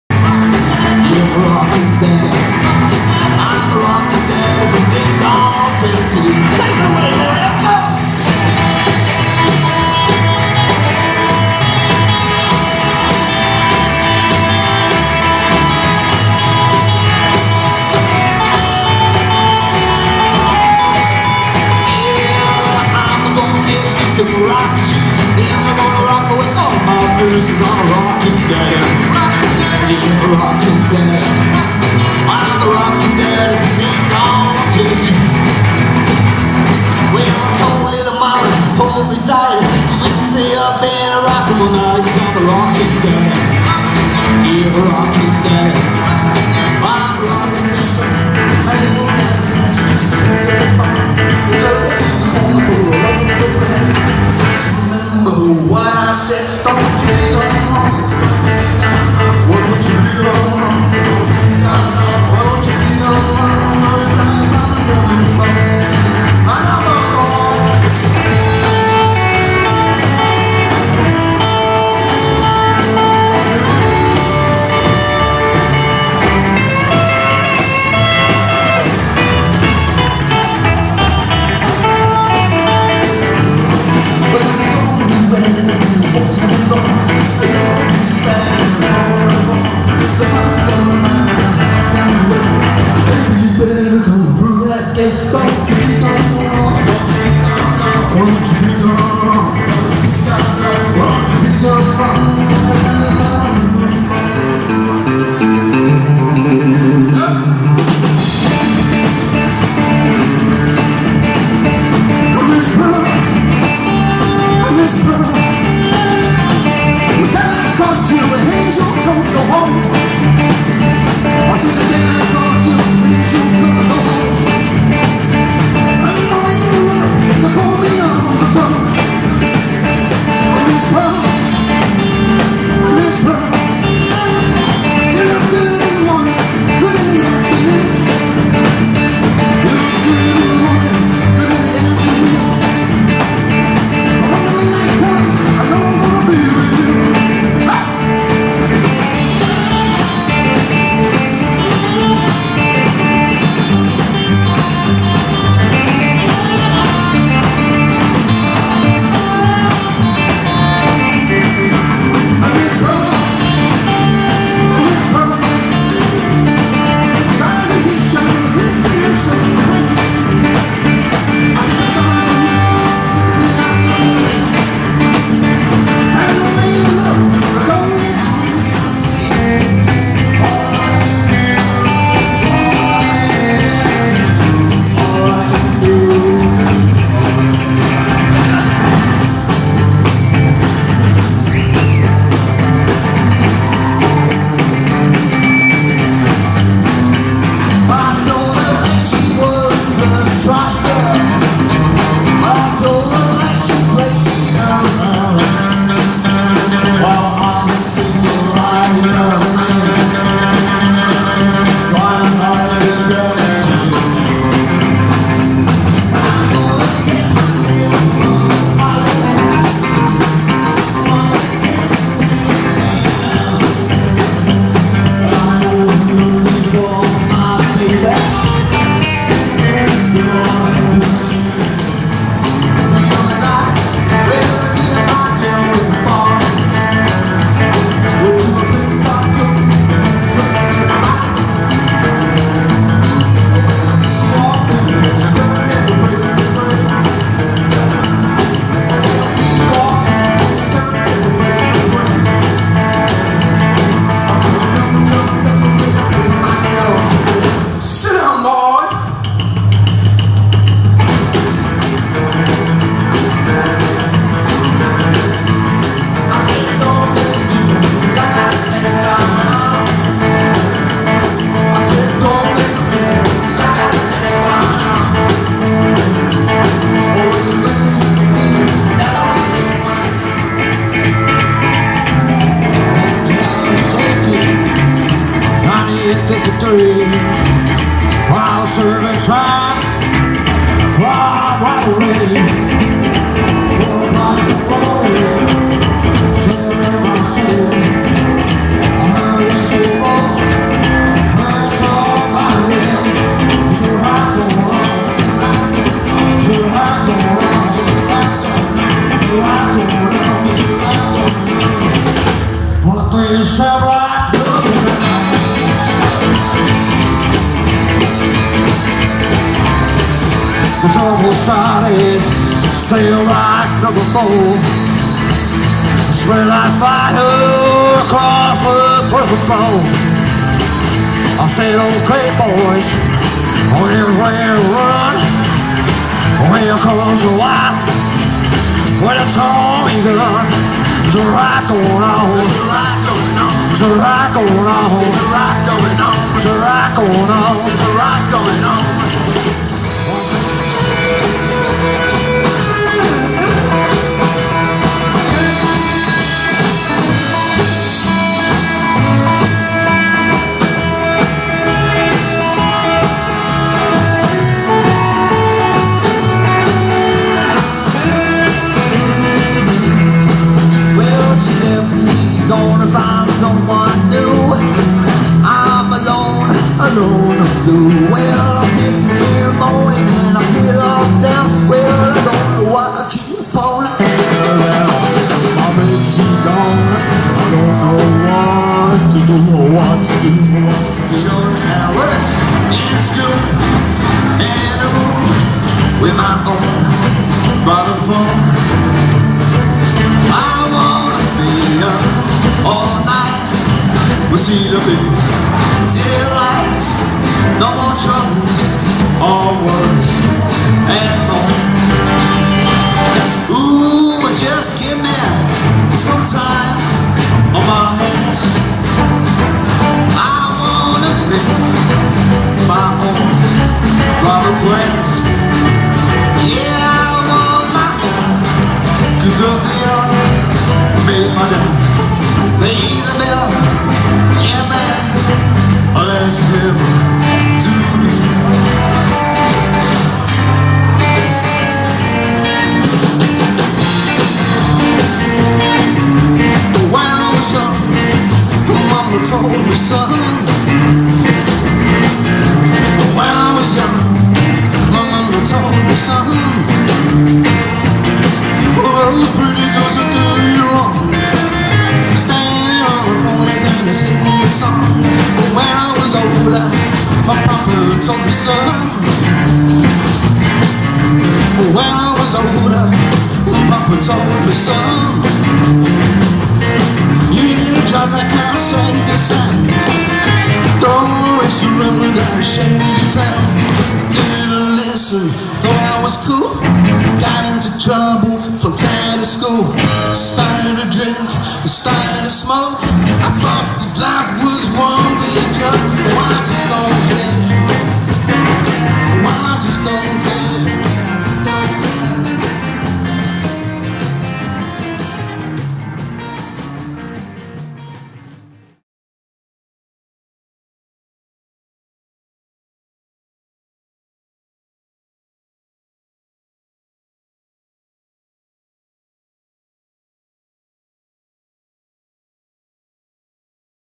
The Roadhouse Roosters - A Montage
A first outing from a new Swindon based band, recently signed to Western Star Records, appearing at Nine Lives Rock n Roll Club in Chelmsford. a short video montage ;-)